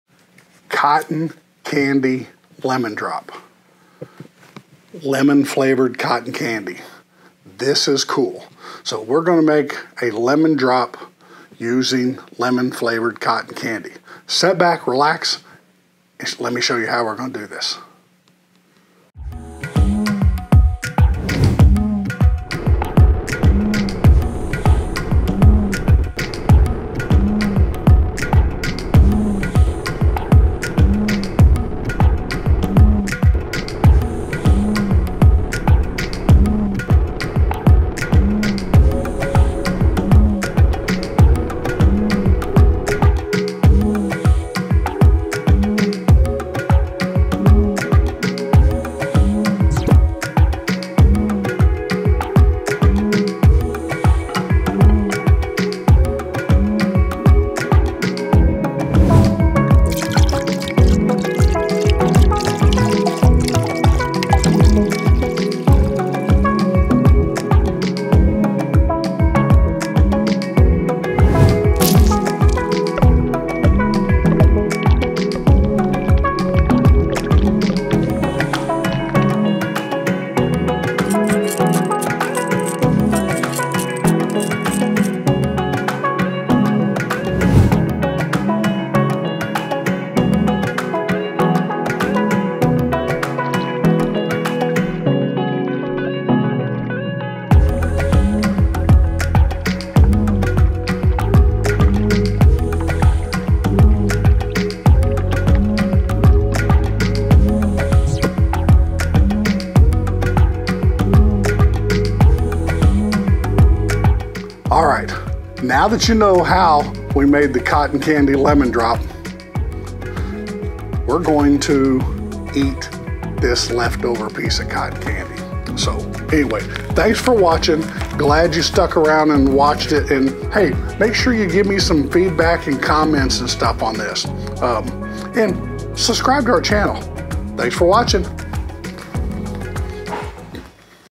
Lemon Drop Martini -- with COTTON CANDY
Today's video is a fun and simple creative twist on a great cocktail, the Lemon Drop martini. As the title suggest, we're adding lemon flavored cotton candy to the cocktail. It's easy to make, and we'll show you how!
Instructions
-Rim the martini glass with lemon rimming sugar
-Make or buy some lemon flavored cotton candy
-Place the cotton candy inside the rimmed martini glass
In a shaker...
-Add ice
-1/2 oz of Paula's Lemon Liqueur
-2 oz of Vodka
-1 oz of freshly squeezed lemon juice
-Shake it and pour over cotton candy filled glass
-Garnish with a lemon wheel